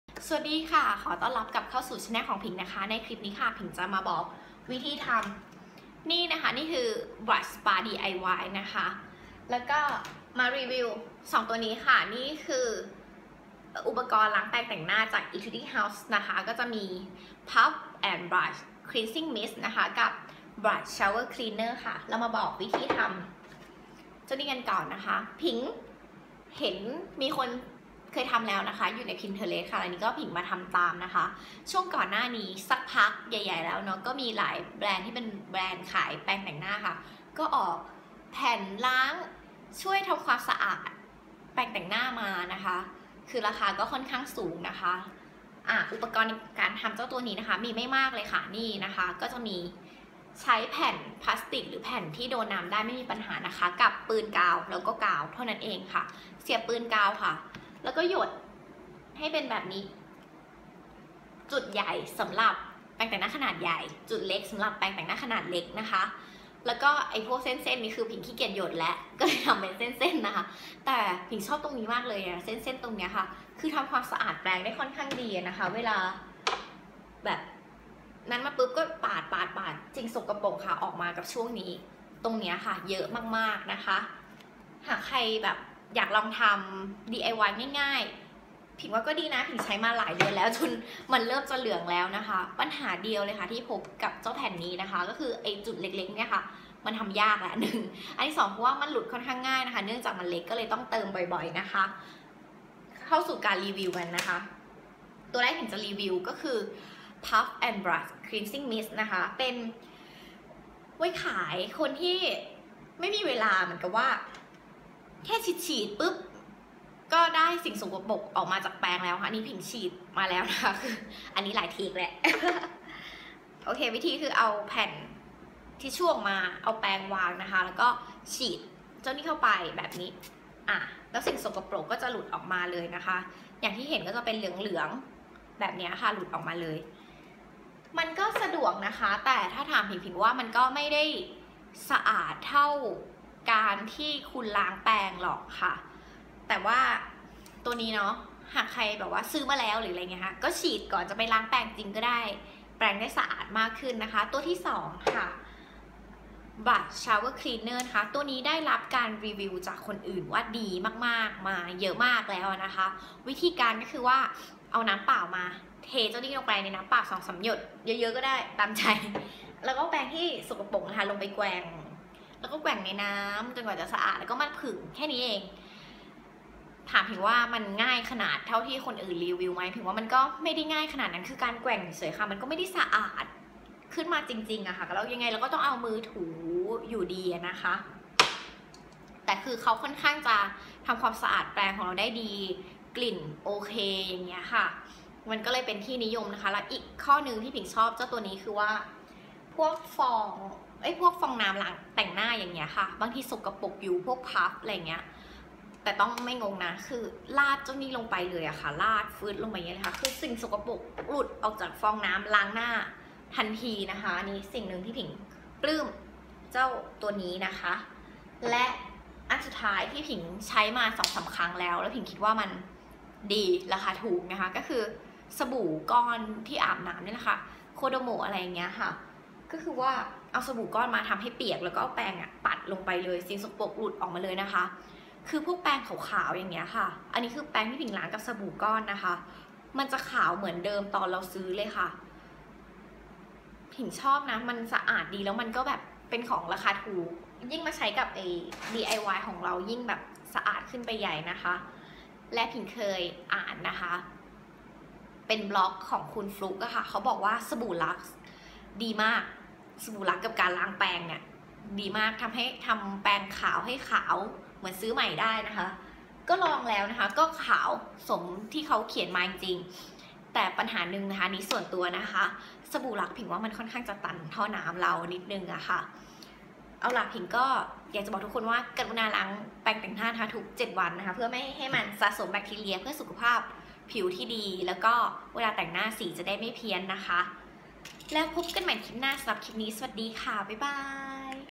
DIY brush spa & review Etude House brush cleaner
DIY brush spa

Etude House
-Puff & Brush cleansing mist
-Brush shower Cleaner

Lerlinping
Lerlinpingstyle